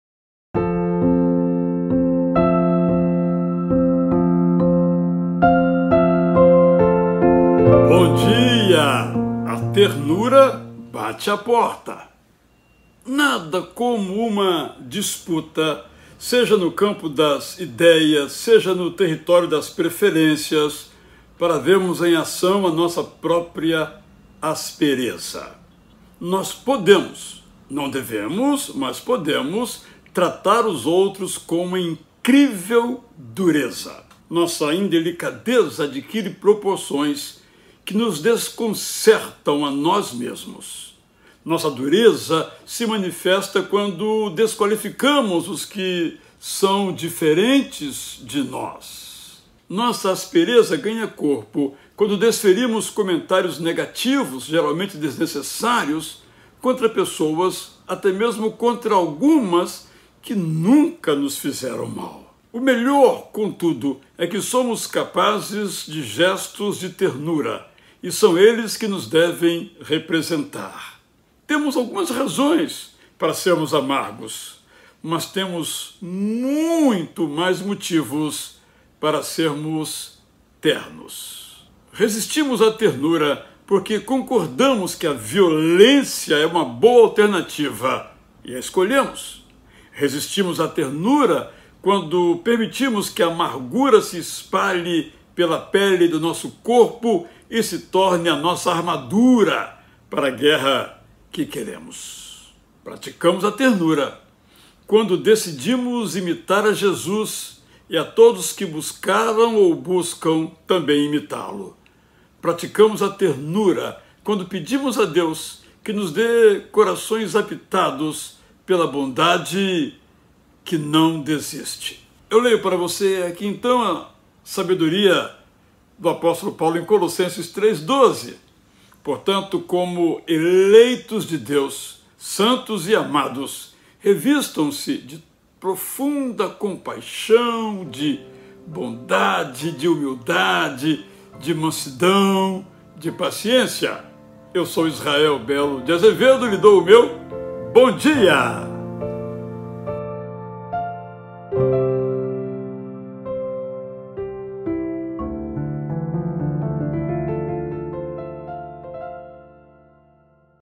(BOM DIA AMIGO, 3220) A ternura bate à porta
“Os homens batem por impotência. Julgam conseguir por meio da força aquilo que não alcançam por meio da ternura e da inteligência”. (Rafael Chirbes, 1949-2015)

A ternura bate à porta

Nada como uma disputa, seja no campo das ideias, seja no território das preferências, para vermos em ação a nossa própria aspereza.
Nós podemos (não devemos, mas podemos) tratar os outros com uma incrível dureza.
Nossa indelicadeza adquire proporções que nos desconcertam a nós mesmos.
Nossa dureza se manifesta quando desclassificamos os que são diferentes de nós.
Nossa aspereza ganha corpo quando desferimos comentários negativos, geralmente desnecessários, contra pessoas, até mesmo contra algumas que nunca nos fizeram mal.
O melhor, contudo, é que somos capazes de gestos de ternura. E são eles que nos devem representar.
Temos algumas razões para sermos amargos, mas temos muito mais motivos para sermos ternos.
Resistimos à ternura porque concordamos que a violência é uma boa alternativa e a escolhemos.
Resistimos à ternura quando permitimos que a amargura se espalhe pela pele do nosso corpo e se torne a nossa armadura para a guerra que queremos.
Praticamos a ternura quando decidimos imitar a Jesus e a todos que buscaram ou buscam também imitá-lo.
Praticamos a ternura quando pedimos a Deus que nos dê corações habitados pela bondade que não desiste.

“Portanto, como eleitos de Deus, santos e amados, revistam-se de profunda compaixão, de bondade, de humildade, de mansidão, de paciência”. (Colossenses 3.12)

Bom dia!